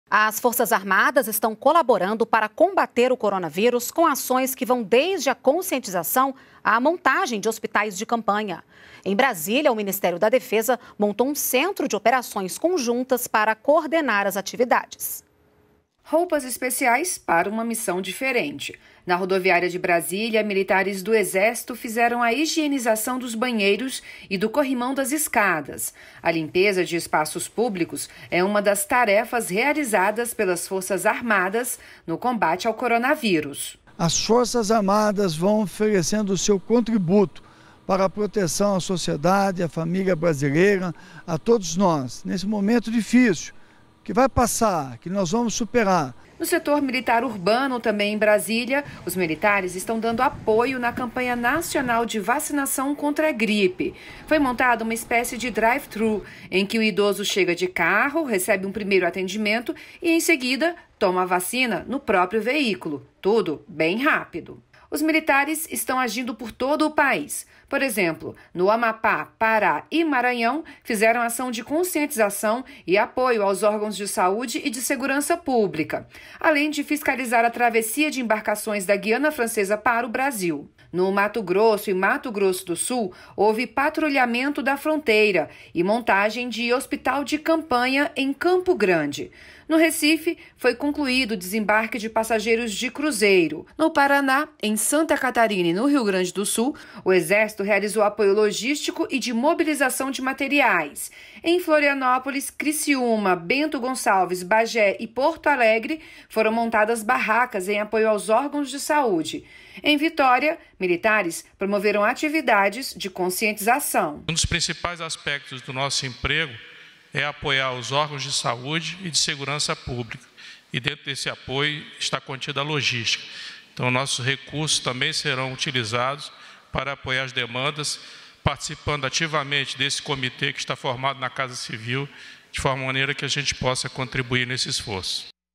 Forças Armadas atuam no combate ao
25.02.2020 - As Forças Armadas estão colaborando para combater o coronavírus com ações que vão desde a conscientização à montagem de hospitais de campanha. Em Brasília, o Ministério da Defesa montou um centro de operações conjuntas para coordenar as atividades.